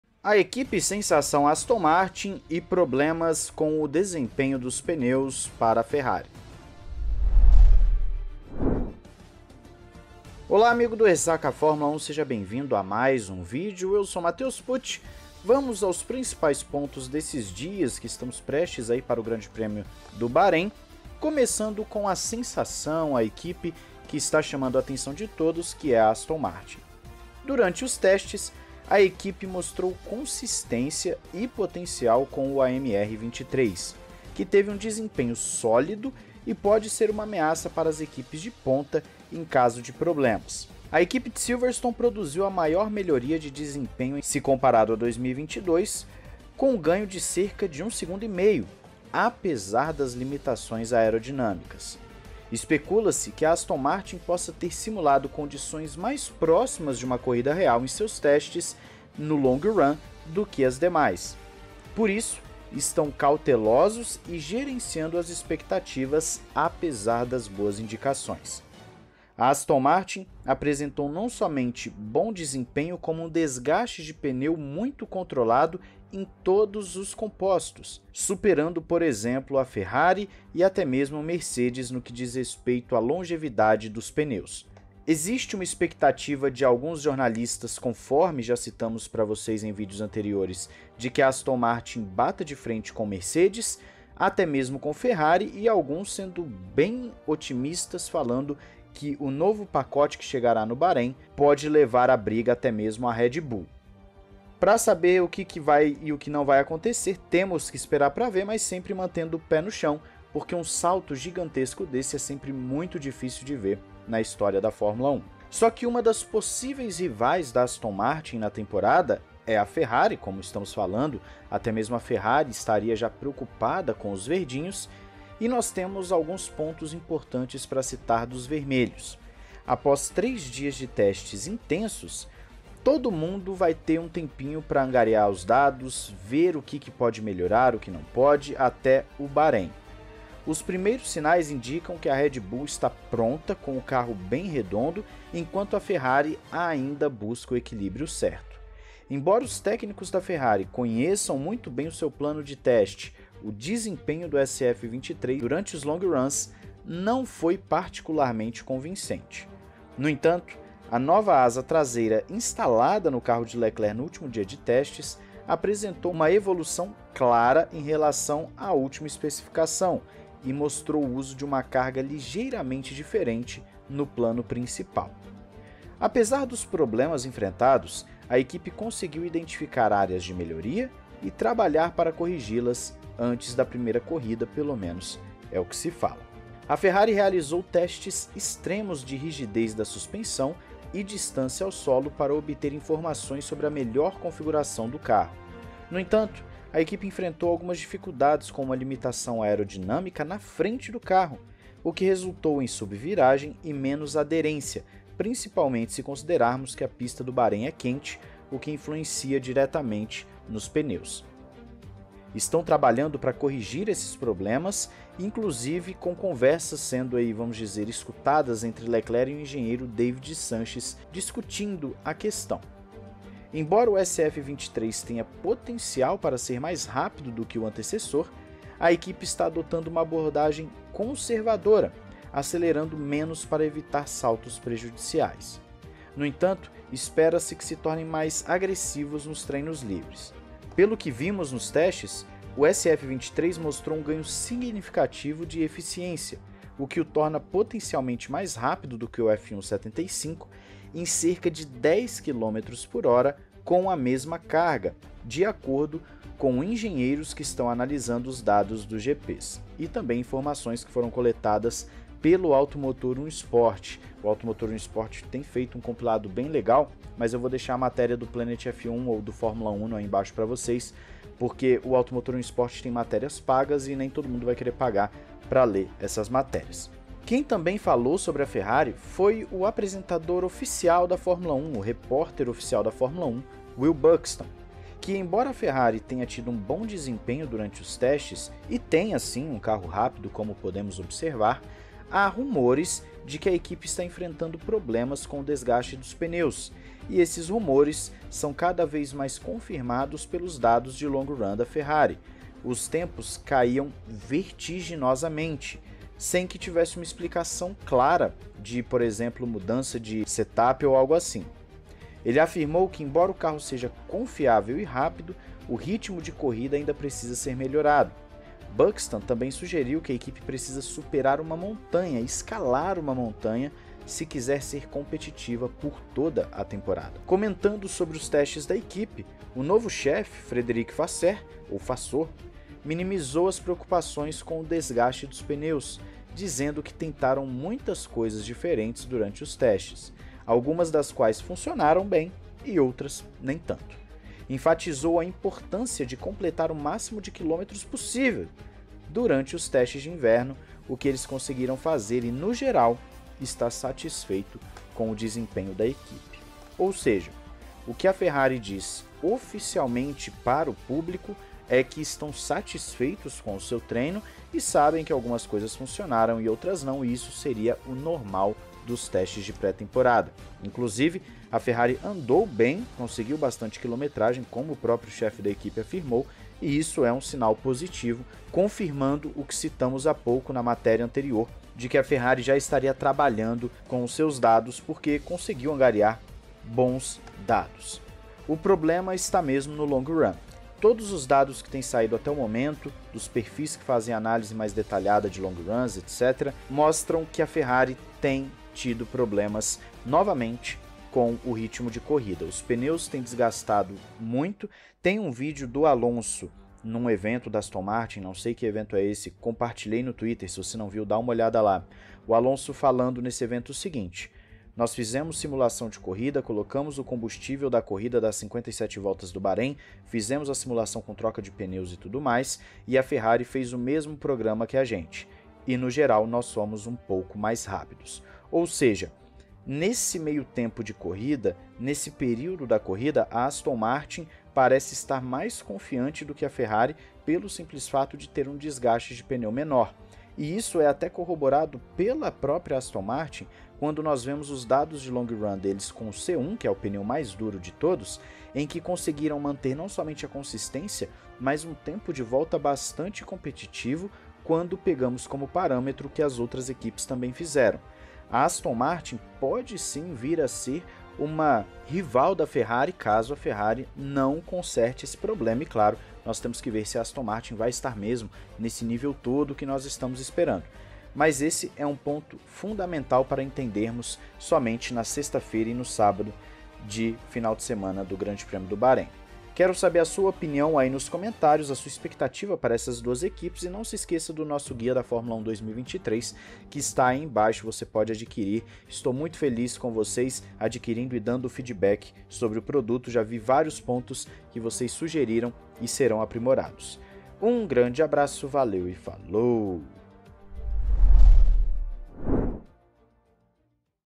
DADOS INICIAIS DA FERRARI INDICAM GRANDE PROBLEMA COM DESGASTE E ALONSO AFIRMA TER SIDO MAIS RÁPIDO
Sergio Milani
@scuderiamilani (Twitter)

João 3:16

00:00 - Introdução
00:21 - Aston Martin 1,5s mais rápida, mas cautelosa
01:56 - Dados iniciais da Ferrari: equilíbrio, asa, pneus
04:00 - SF23 Mais eficiente que o F1-75
04:37 - Buxton cita grande diferença para Red Bull
05:27 - Vasseur minimiza preocupações
06:28 - Long Runs ruins da Ferrari e Aston mais rápida